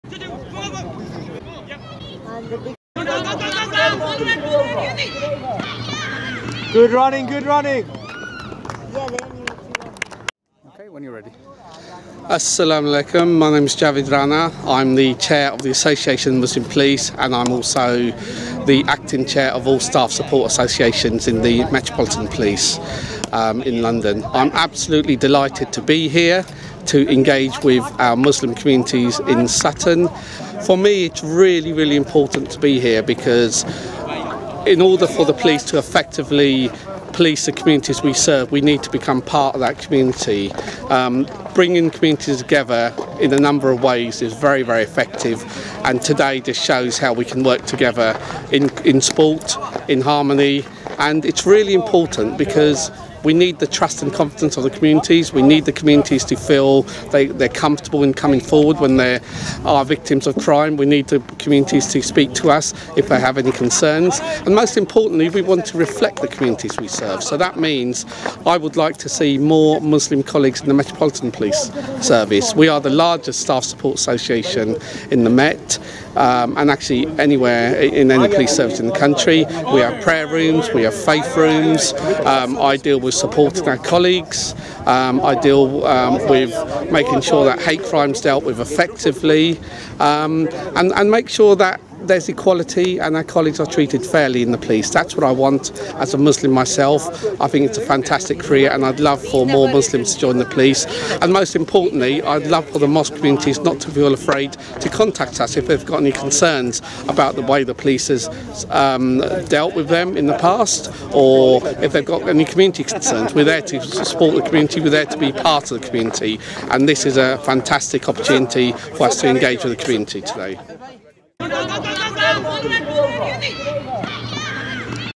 Javid Rana - Chair, Association of Muslim Police MPS | T20 Community Cricket Match 2019
Thursday 1st August 2019